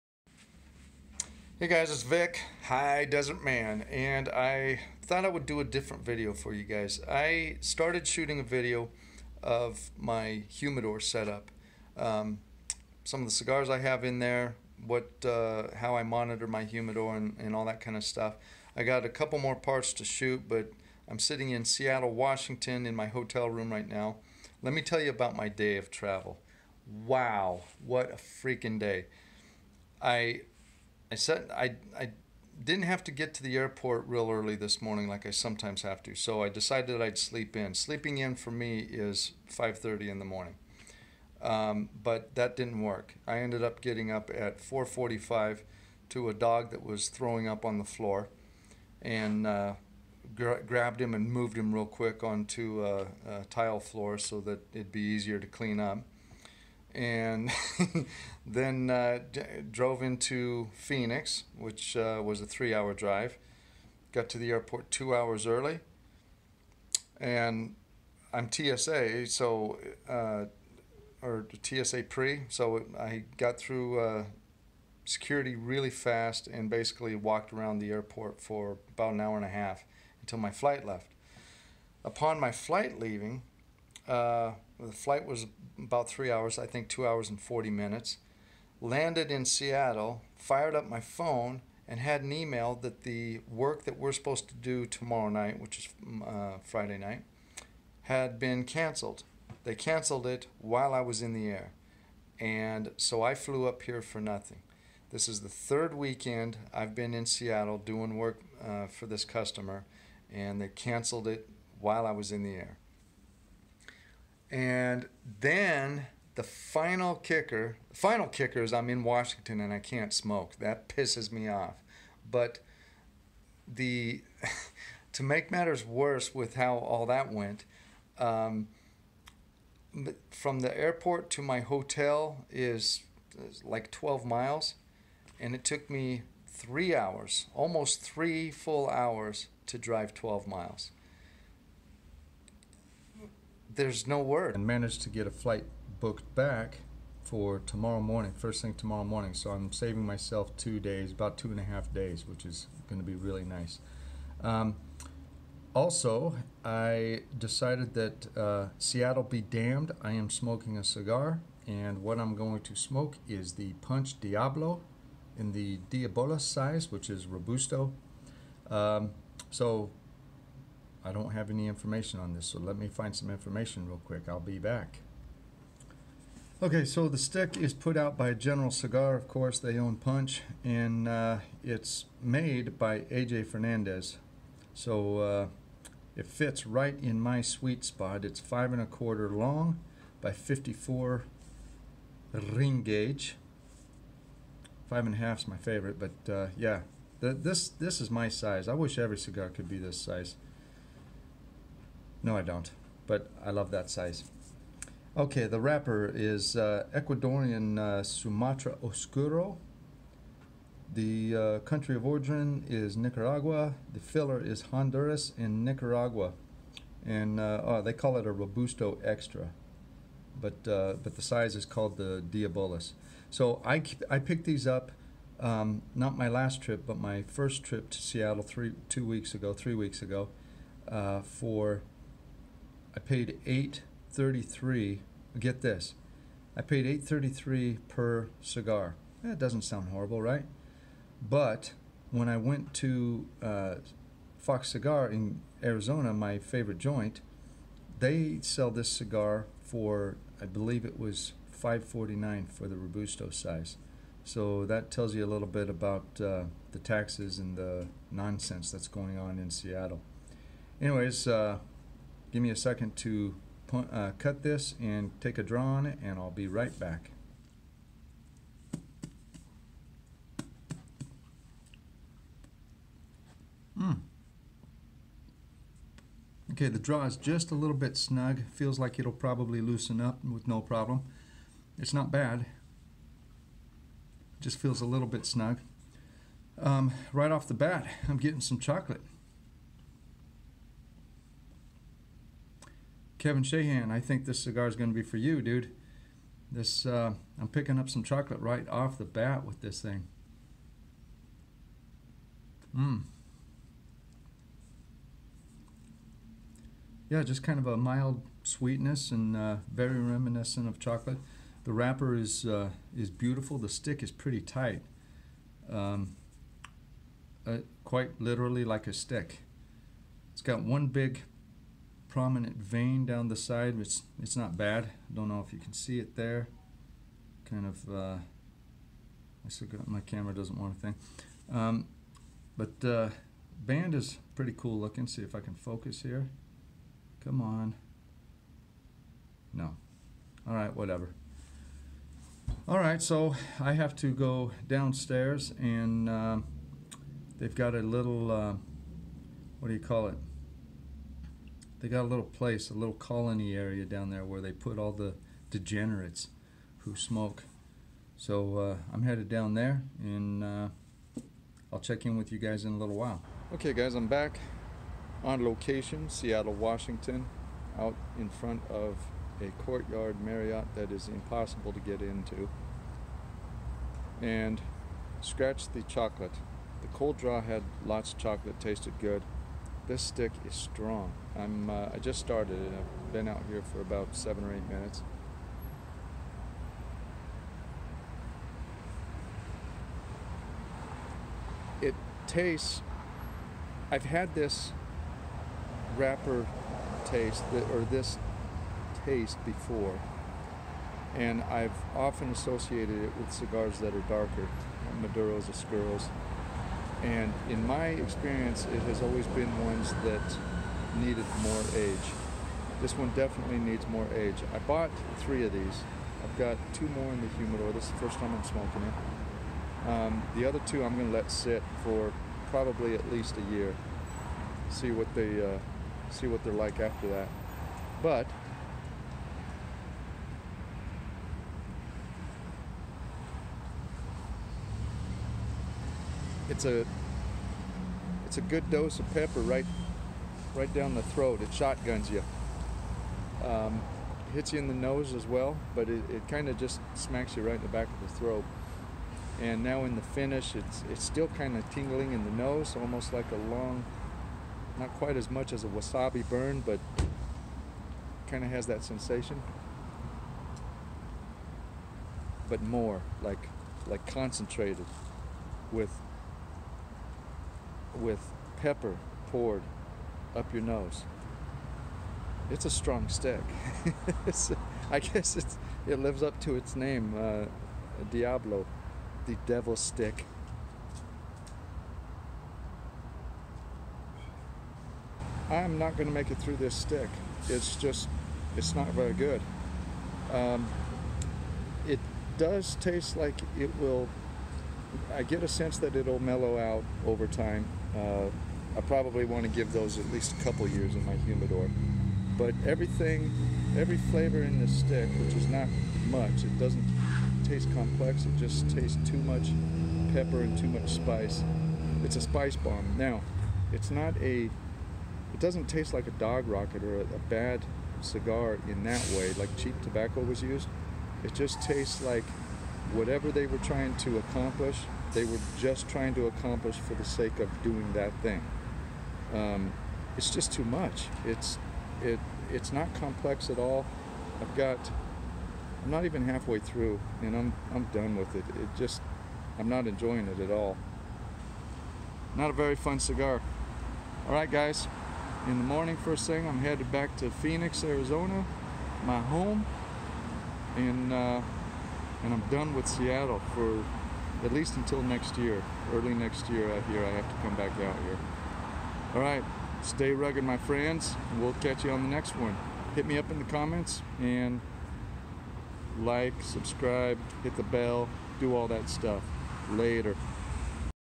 A bad Punch Diablo in Seattle
Are 3 trips in a row enough to make a determination about a place...?

This stick comes to us from AJ Fernandez via General Cigar. That's all I can say about it.